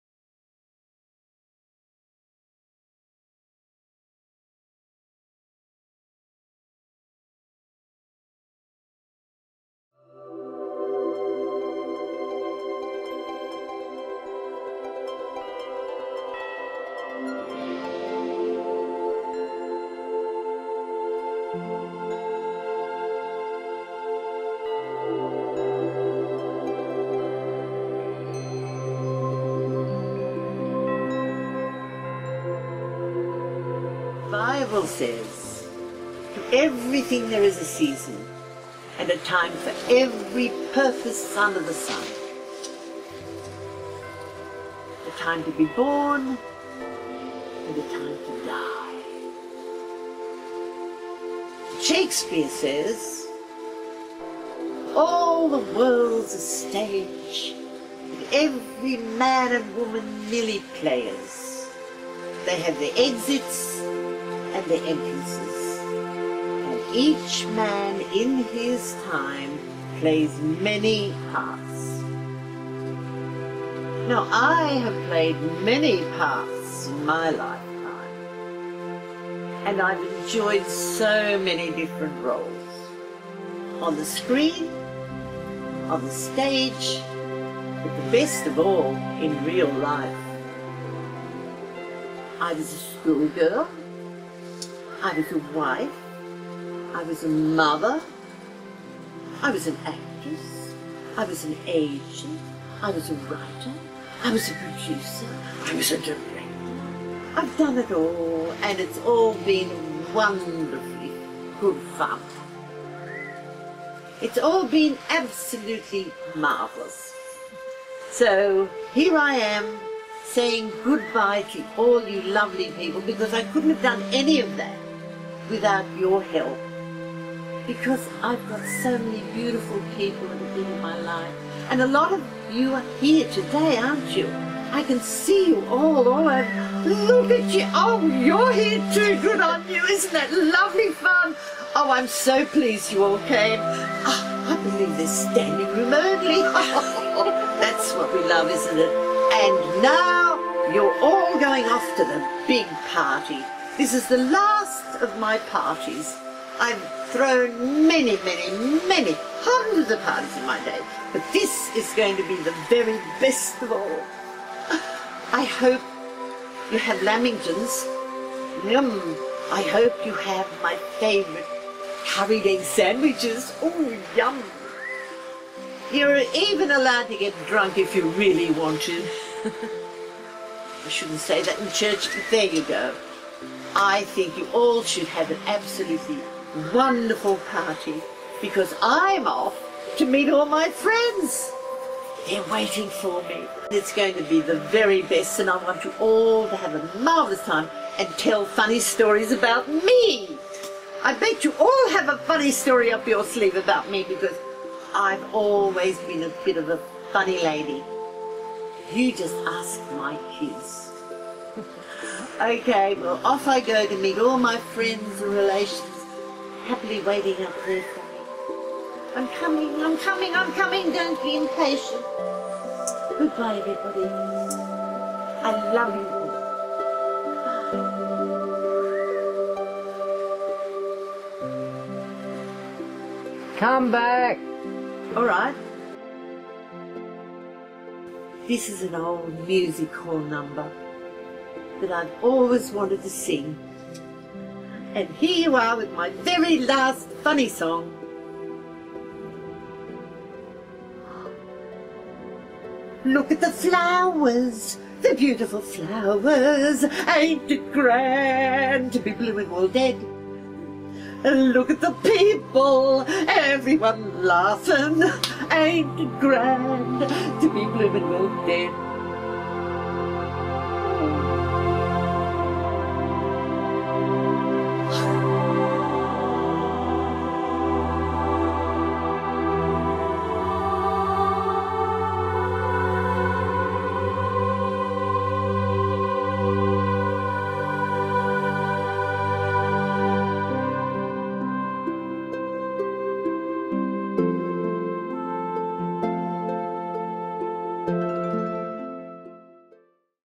The Spectacular Farewell of Joy Hruby
My wonderful, unique and kind mother stage managed her own funeral completely, right down to making a farewell video that made it impossible not to celebrate her life in lieu of mourning her death. She would have been delighted to know that the day after her funeral this video was celebrated on prime time television. Enjoy!